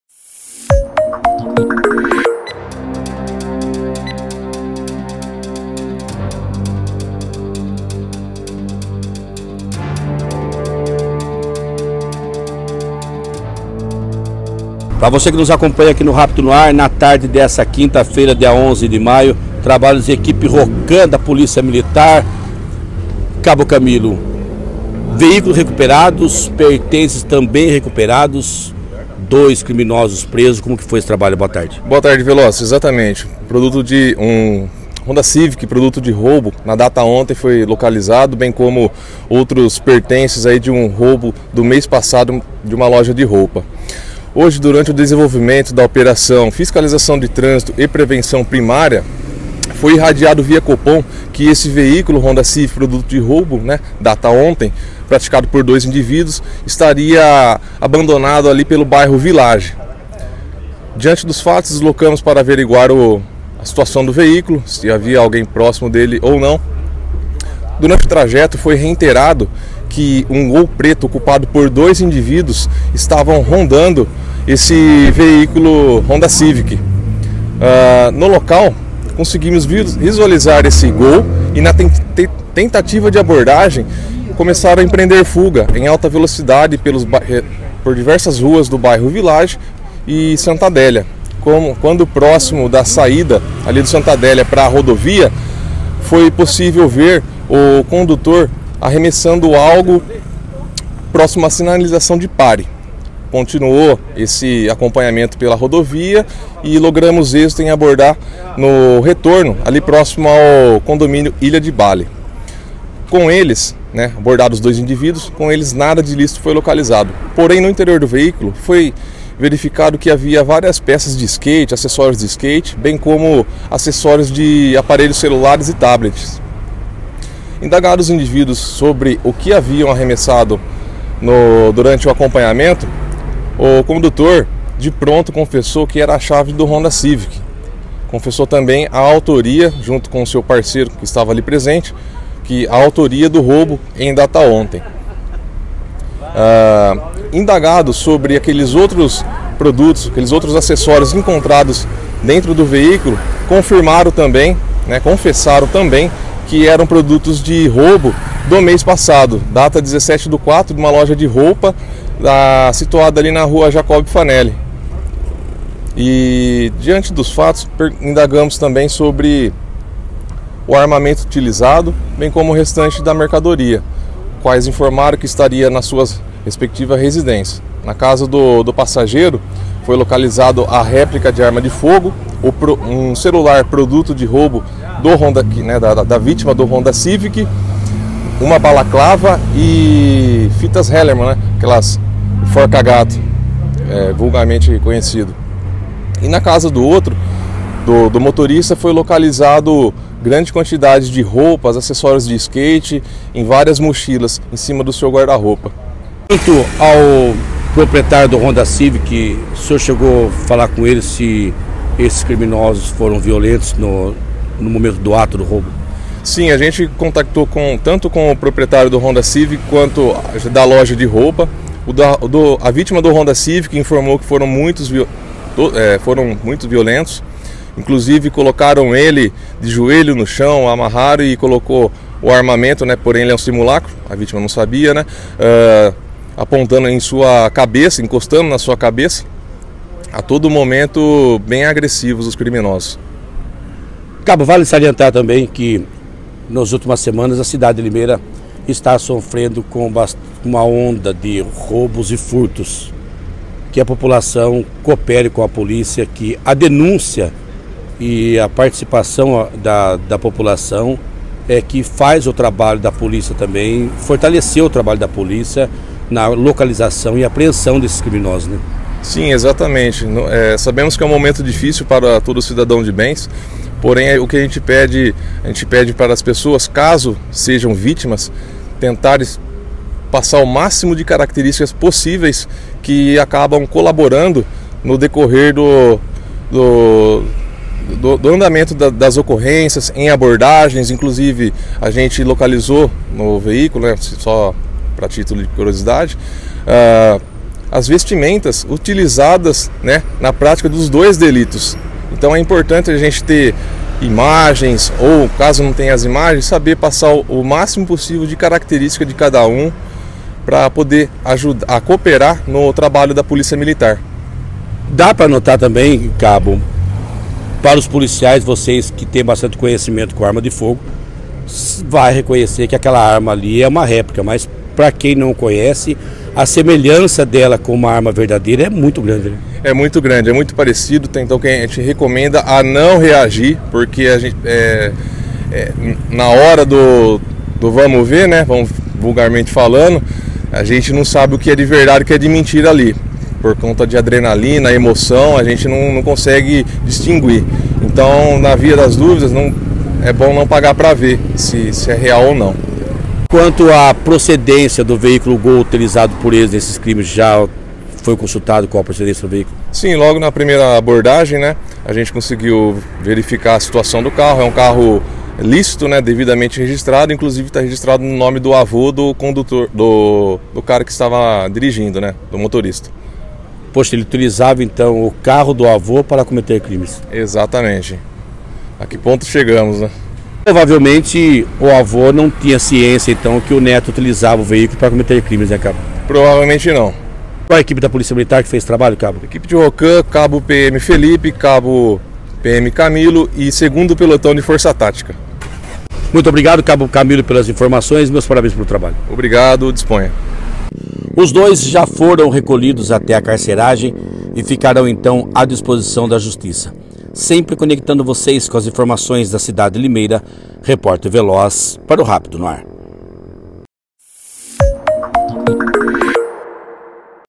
Polícia Militar captura dupla envolvida em assaltos em Limeira
Dois criminosos, um de 18 e outro de 19 anos, foram presos na tarde desta quinta-feira (11) em Limeira (SP), após uma ação da Polícia Militar. A prisão ocorreu durante uma fiscalização de trânsito e prevenção primária, quando os policiais avistaram um veículo suspeito no Jardim Residencial Village.